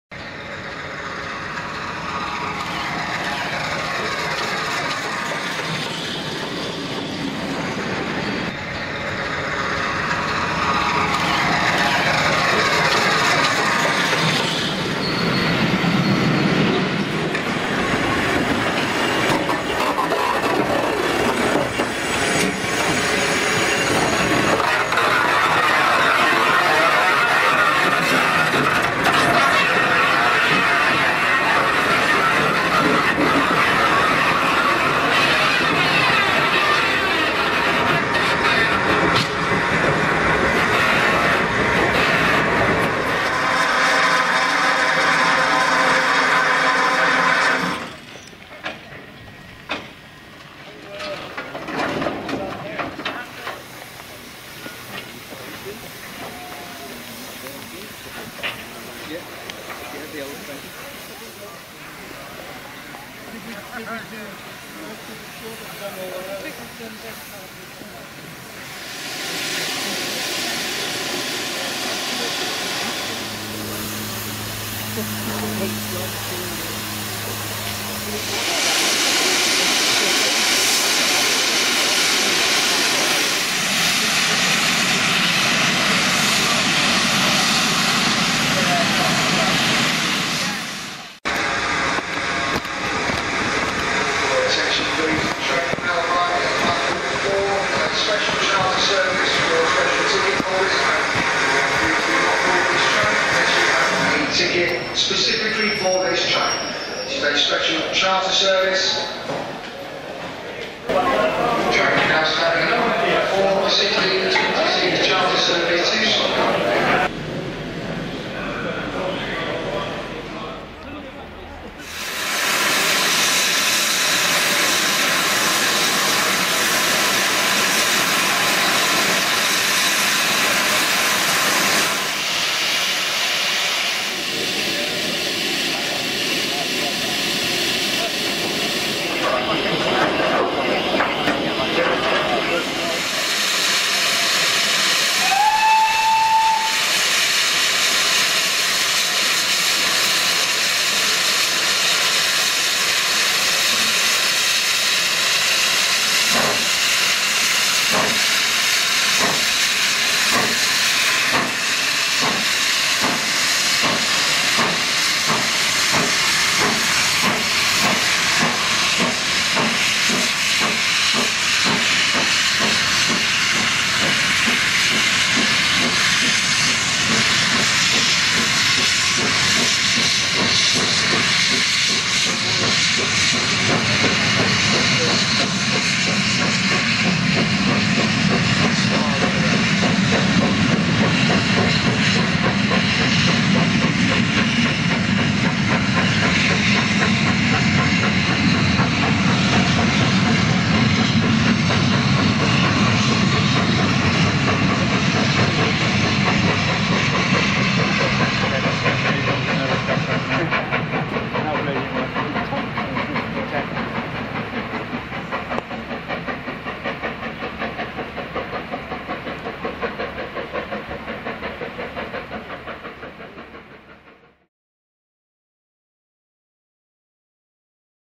The Moonraker - 5043 - Solihull to Sailsbury - 08/04/17
Gone are the days of open window recordings. However here are some of the pictures I was able to make on a wonderful run organised by Vintage Trains from Solihull to Sailsbury and back with GWR Castle Class 5043 Earl of Mount Edgcumbe hauling 'The Moonraker'.  Recorded locations were at Solihull, Didcot Railway Centre and Sailsbury – and Banbury on the return journey. The memorable moments of this trip was the return non stop run between Sailsbury tp Didcot and climbing of Hatton bank. The final location was the departure from Solihull with empty stock returning to Tyseley at the end of  the glorious summer’s day Saturday 8th April.2017.